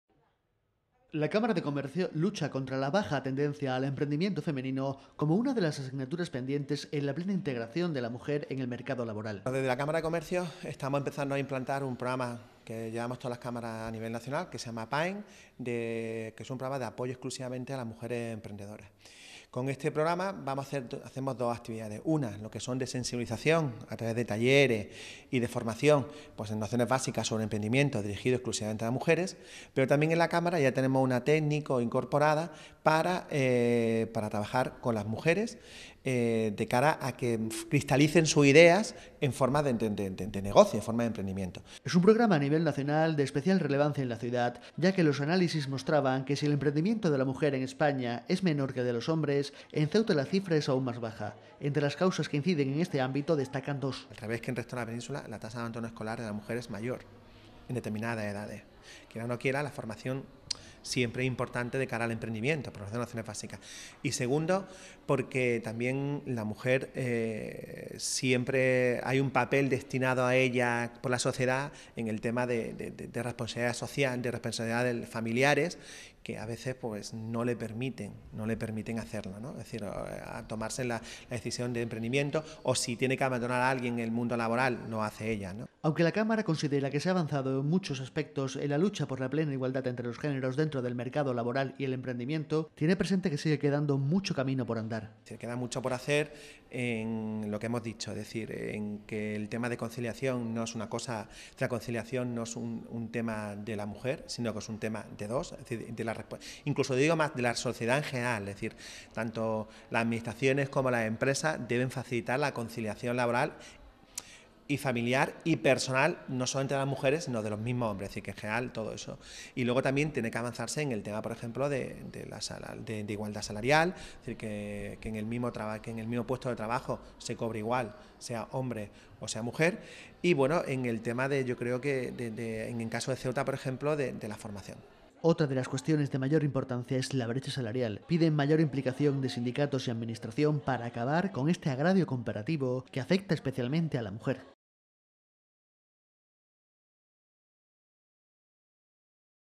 La Cámara de Comercio apoya al emprendimiento femenino, deficitario en Ceuta
La Cámara de Comercios de Ceuta, en consonancia con las cámaras nacionales, está desarrollando medidas de apoyo al emprendimiento femenino. Es una iniciativa en respuesta a los análisis que muestran el bajo índice en Ceuta, menor que en el resto de España, de por sí bajo. Esta, unida a la brecha salarial o el abandono escolar, son algunas de las áreas que mas necesidad de actuaciones tiene.